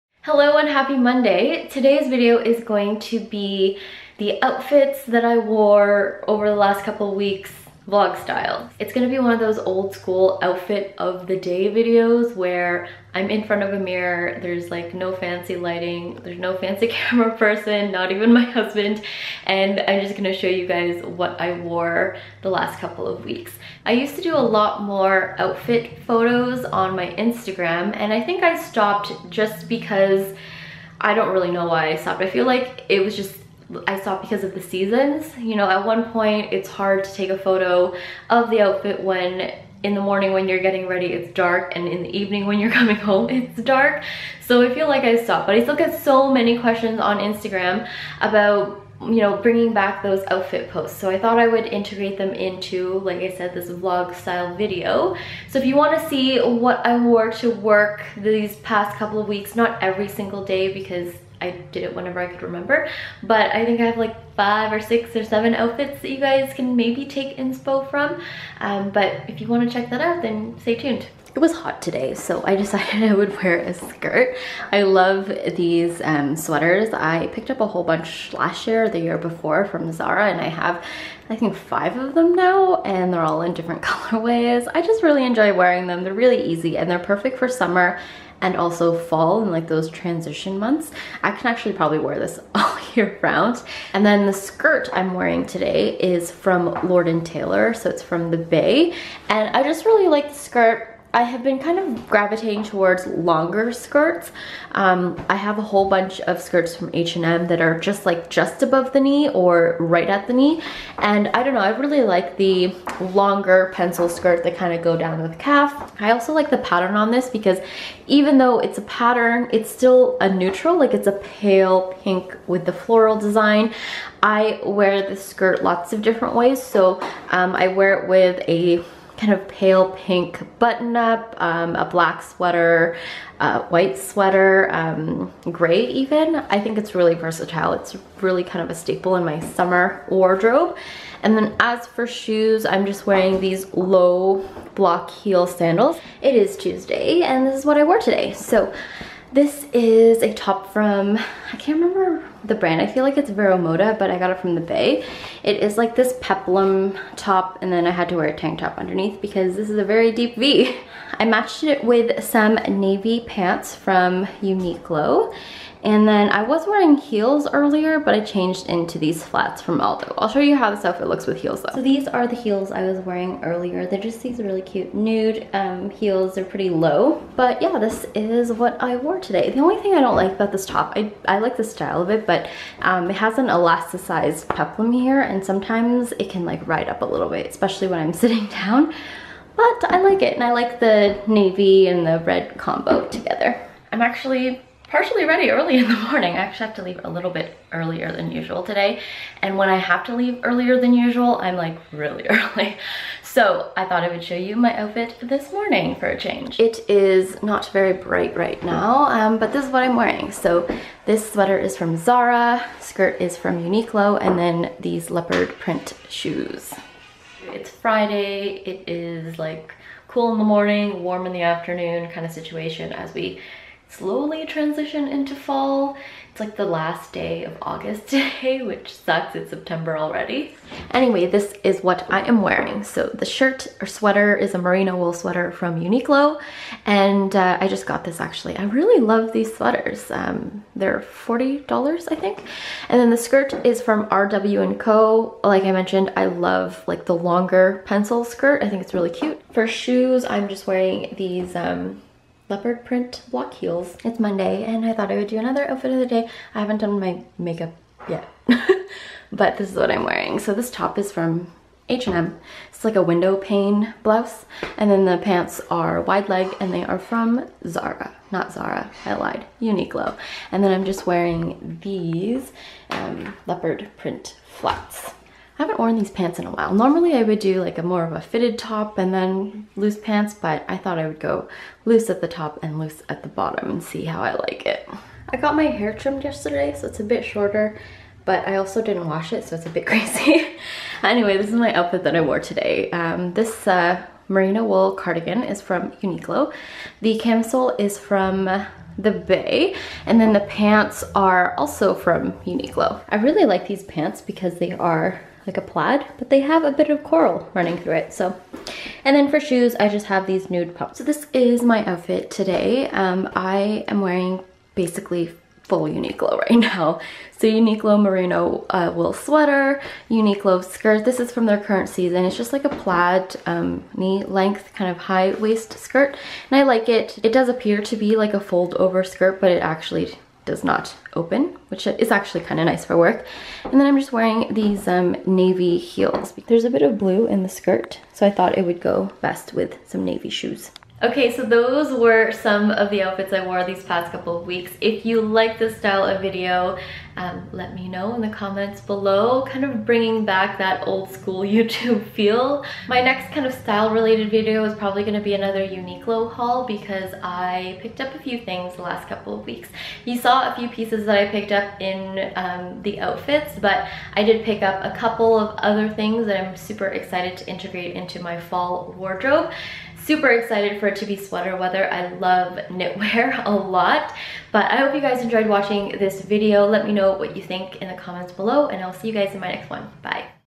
Outfits of the Week | September 2018
The one where I share some work outfits.

Mentioned//

1:12 Outfit #1:
Lord & Taylor Pencil Skirt (last season)
Block Heels (very old)

2:26 Outfit #2:
Vero Moda Peplum Top
Aldo nude flats / Christian Louboutin Pigalle 55mm

3:13 Outfit #3
Calvin Klein Leopard print block heels (from the Bay)

3:37 Outfit #4
RW&Co Pencil Skirt (last season)

4:13 Outfit #5:
H&M Windowpane Blouse
Leopard print flats (super old)

4:45 Outfit #6:
Christian Louboutin Pigalle 55mm

5:10 Outfit #7:
Calvin Klein heels